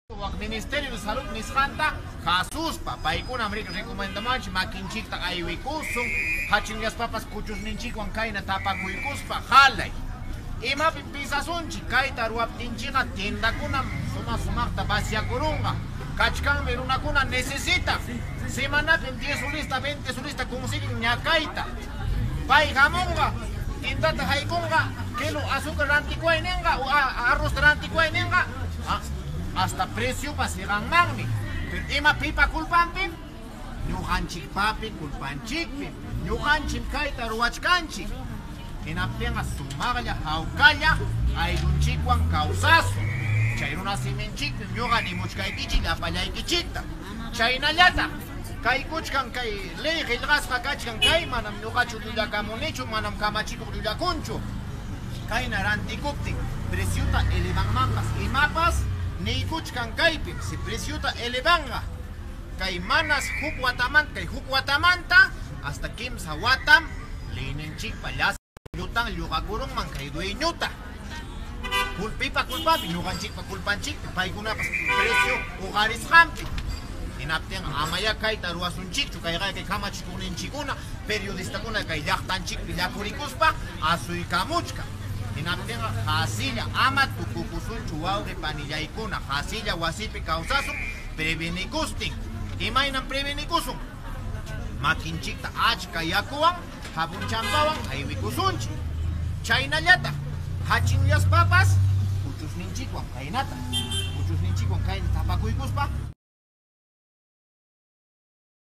Coronavirusmanta policía willamuchkawanchis
RUNASIMIPI Huancavelicamanta policía runasimipi willamuchkawanchis coronavirusta llallinamchispaq.

Policía de Huancavelica da recomendaciones en quechua sobre cómo afrontar el nuevo coronavirus.